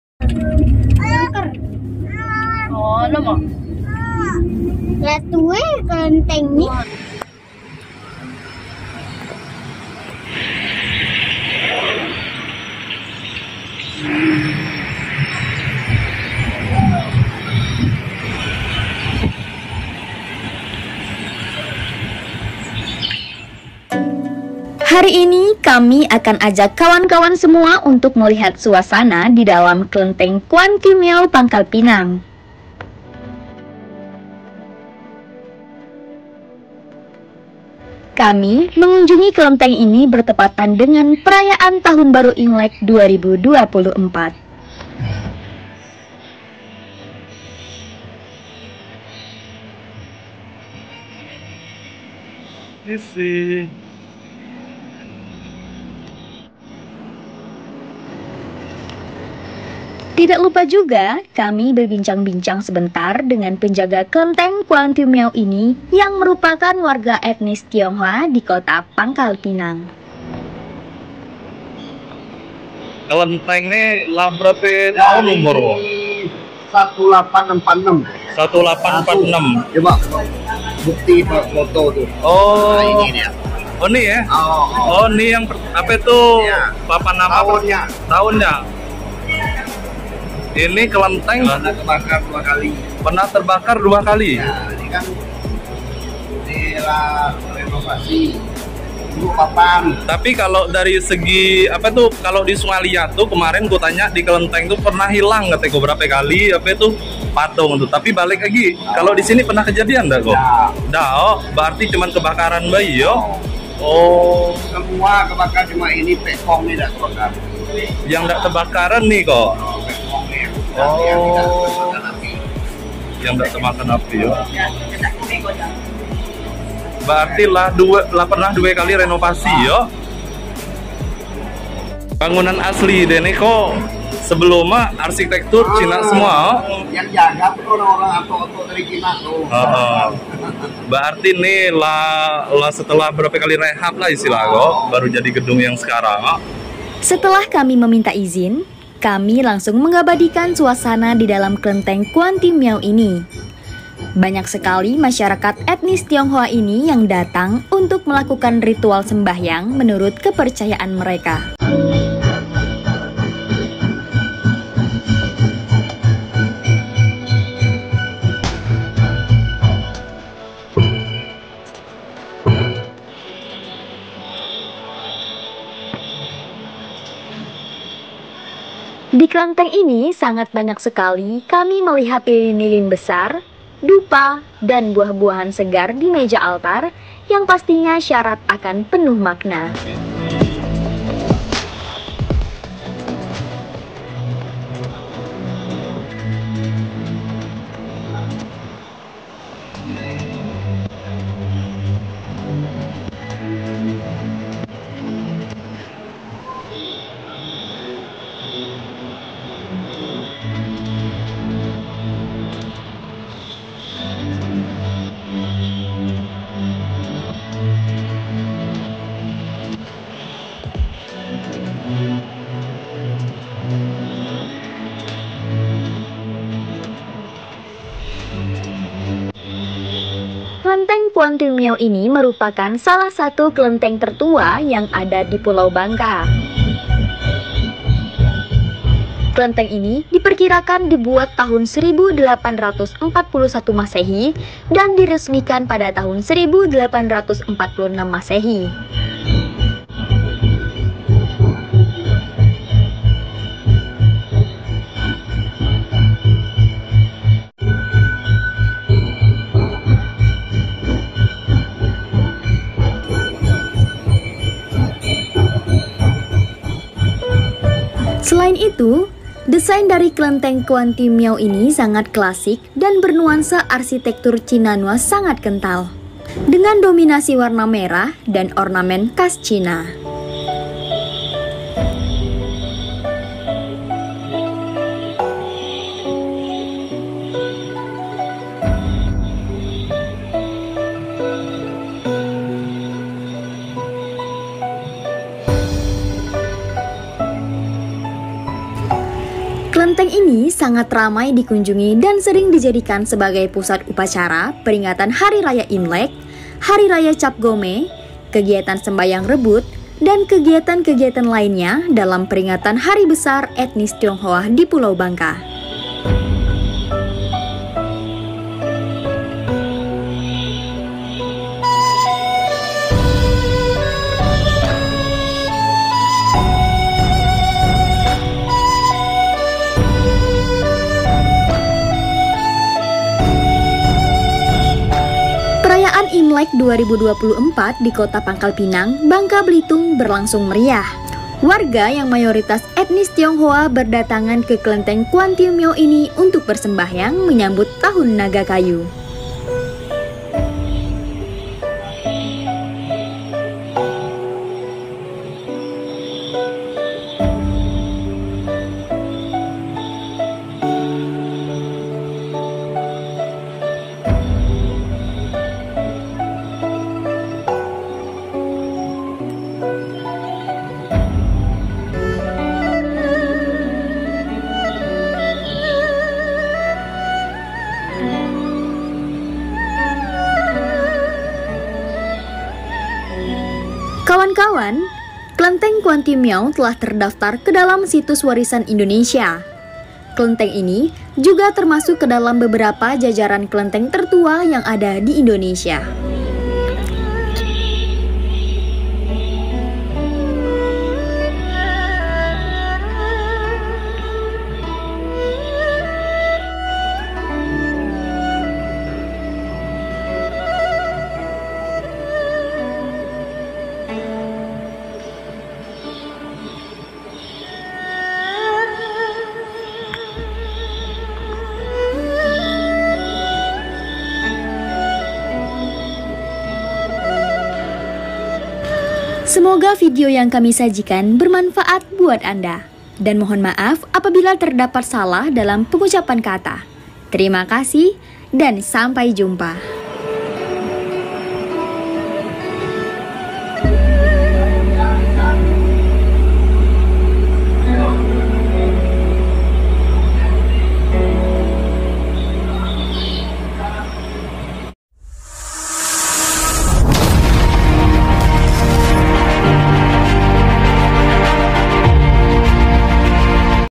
KELENTENG INI BERUSIA RATUSAN TAHUN DI PULAU BANGKA (Kelenteng Kwan Tie Miau)
Kelenteng Kwan Tie Miaw ini merupakan salah satu kelenteng tertua yang ada di Pulau Bangka.
Kelenteng ini diperkirakan dibuat tahun 1841 M dan diresmikan pada tahun 1846 M.
Selain itu,Desain dari Klenteng Kwan Tie Miaw sangat klasik dan bernuansa arsitektur Cinanua sangat kental. Dengan dominasi warna merah dan ornament khas Cina.

Kelenteng ini sangat ramai di kunjungi dan sering di jadikan sebagai pusat upacara peringatan hari raya Imlek,hari raya Cap Go Meh, kegiatan sembahyang Rebut dan kegiatan2 lainya dalam peringatan hari besar etnis Tionghoa di Pulau Bangka.

Imlek/kongian di beberapa wilayah di Bangka Belitung.